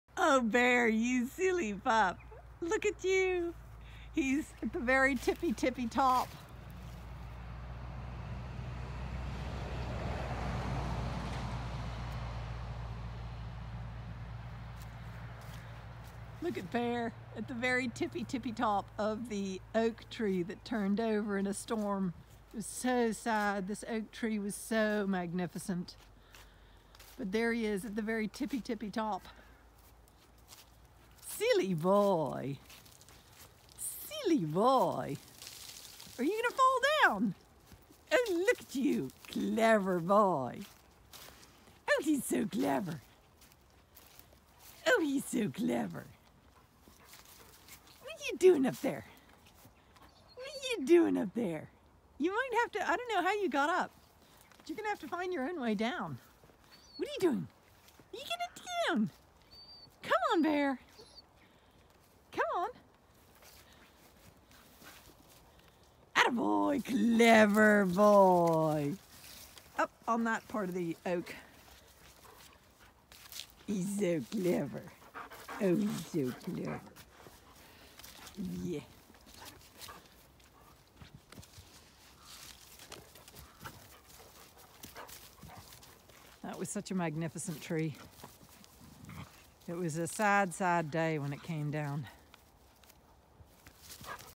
Bear happy climbing even a fallen great oak tree from 2017
No matter the state of a tree Bear loves to climb it